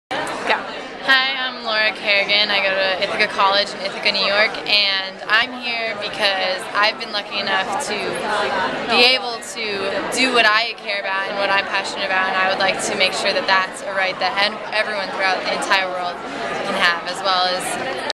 Extend Rights to Everyone
End Genocide Action Summit participants speak out about why they took part in the conference.